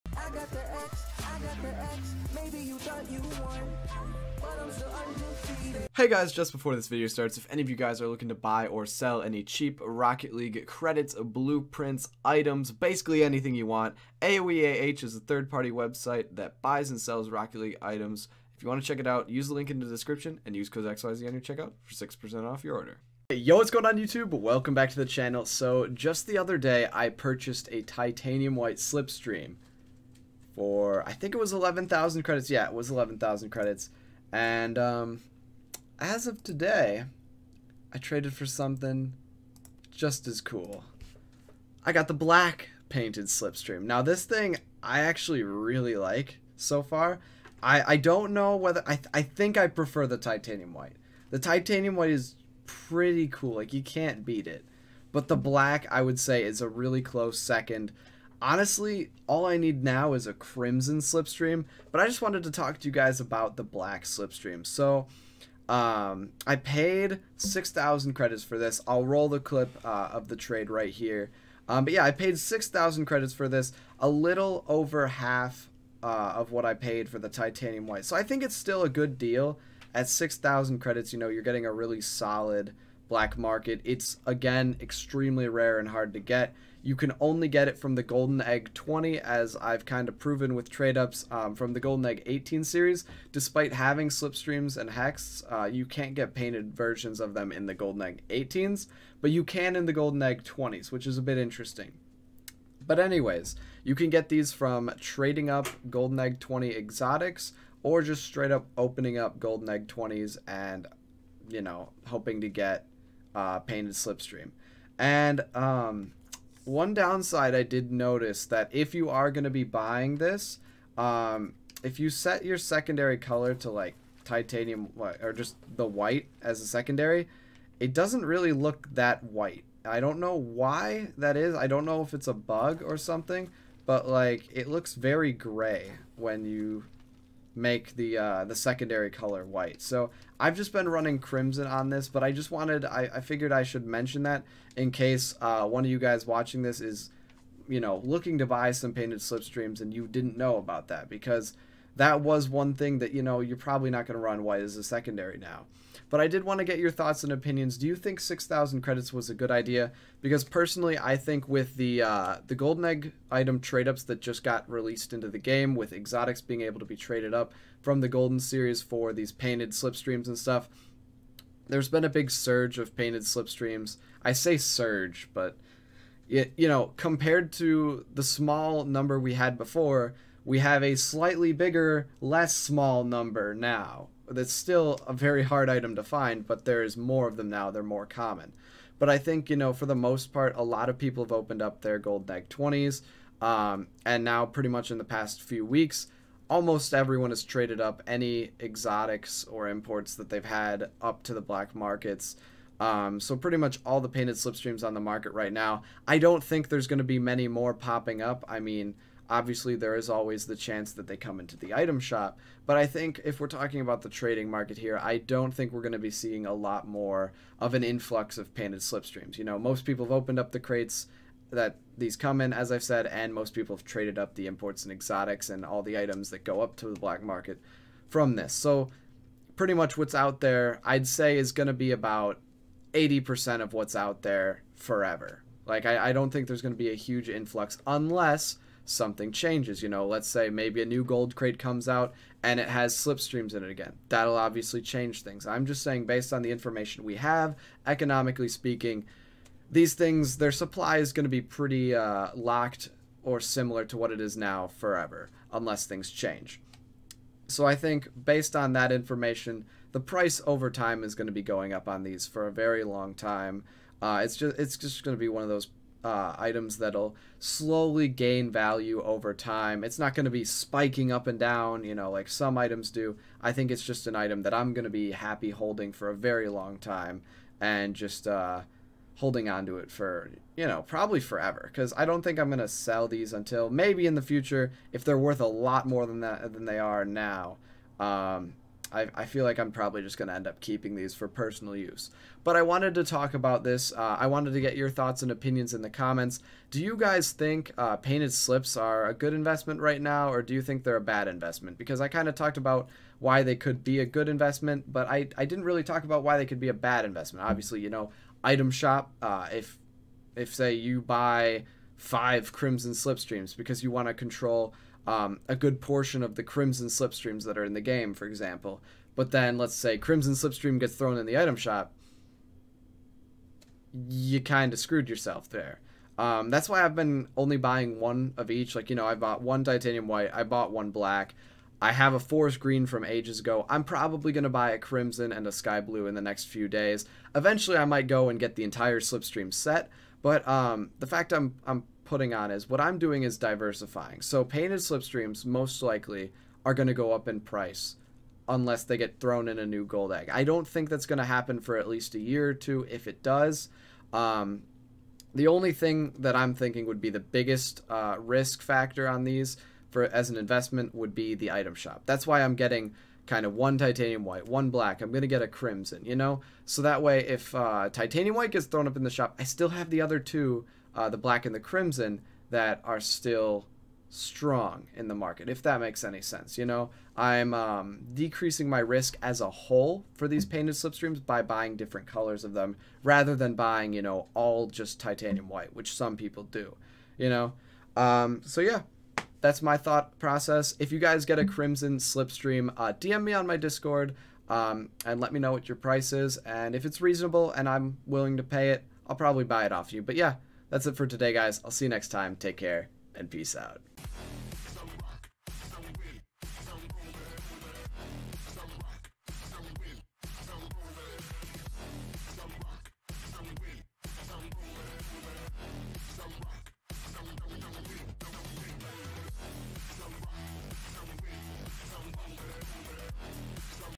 I GOT A BLACK PAINTED SLIPSTREAM!!!
Today we discuss my investment of 6000c into a black slipstream, what do you think of painted slipstreams as an investment?

For Fast, Safe, And Reliable Credits, Items, Gift Cards And More!
FOR DISCOUNT, USE CODE: XYZ